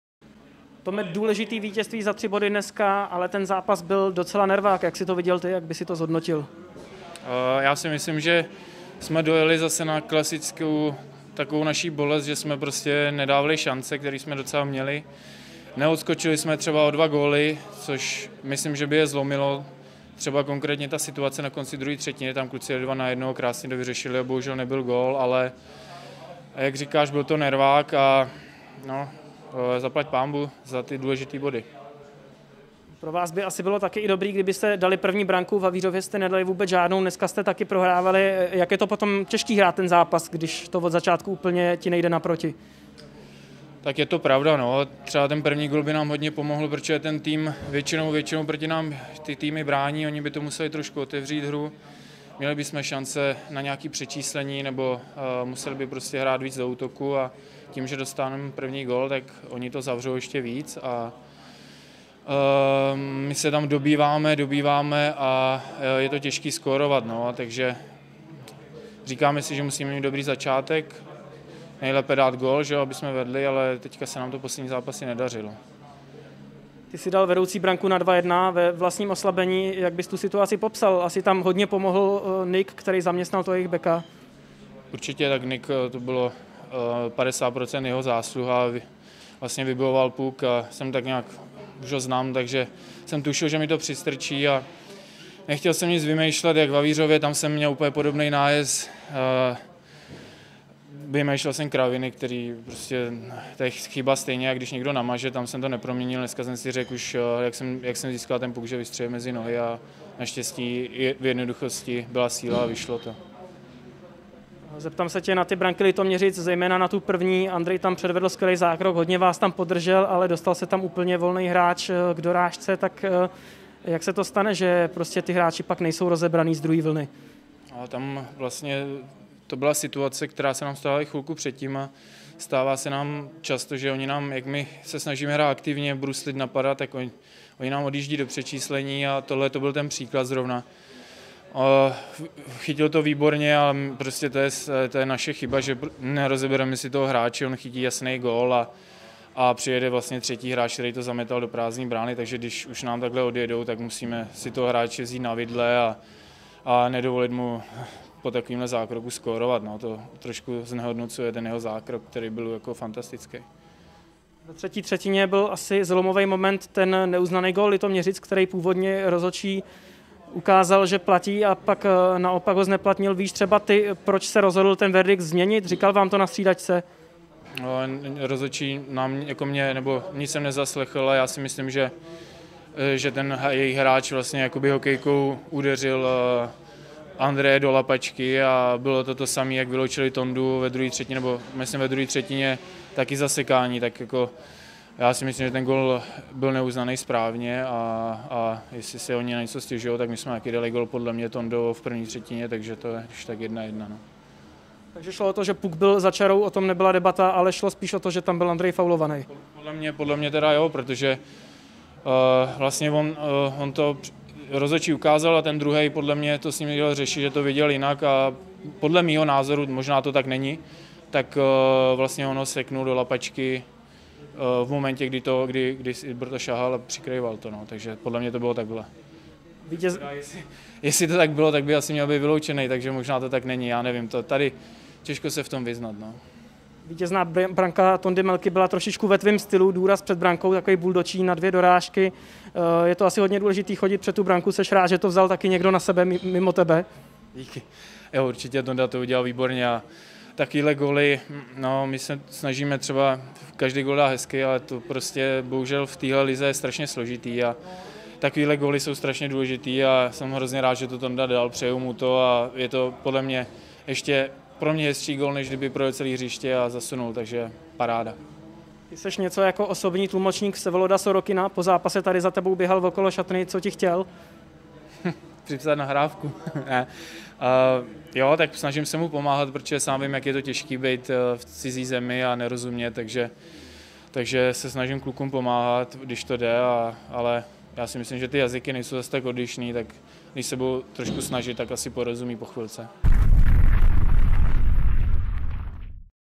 Pitule: Nevyužíváme šance, pak je z toho nervák. Zaplaťpánbůh za body
„Týmy k nám jezdí bránit. Když dostaneme první gól, zavřou to ještě víc, pak je těžké skórovat,“ komentoval drama s Litoměřicemi Tomáš Pitule.